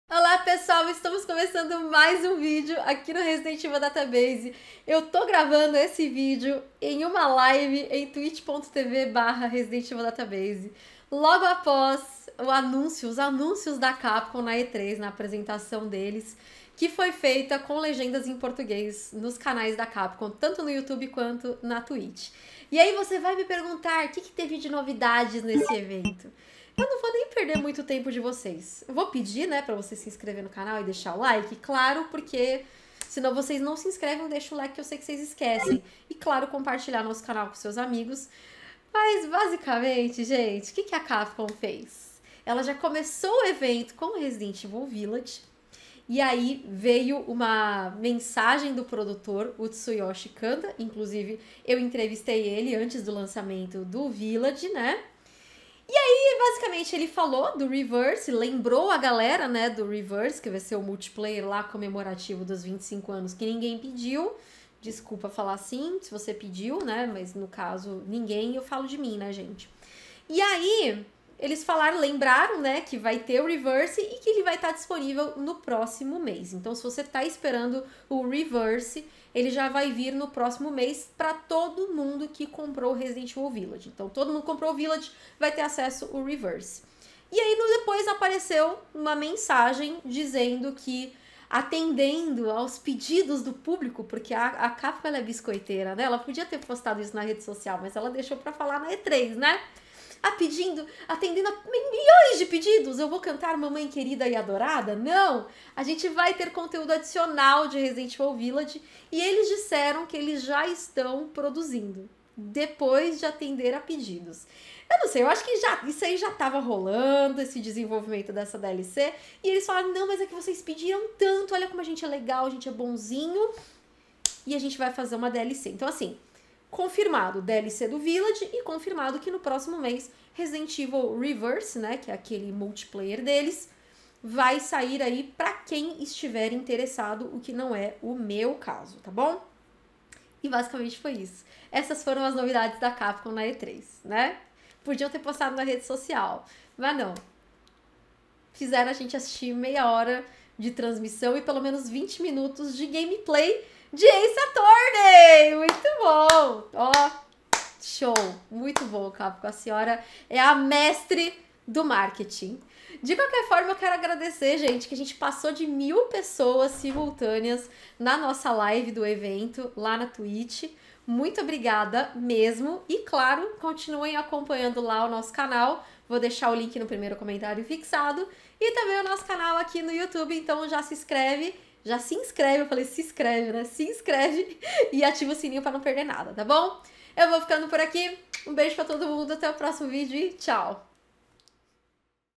O que teve da CAPCOM (e de Resident Evil Village) na E3 2021? 🤡
A Capcom realizou uma apresentação de 30 minutos na E3 2021, e entre os jogos que estavam programados para ter atualização, estava Resident Evil Village! Além de revelarem a chegada do multiplayer RE: Verse para o próximo mês, confirmaram a produção de uma DLC. Saiba os (poucos) detalhes aqui!

💌 FALE CONOSCO!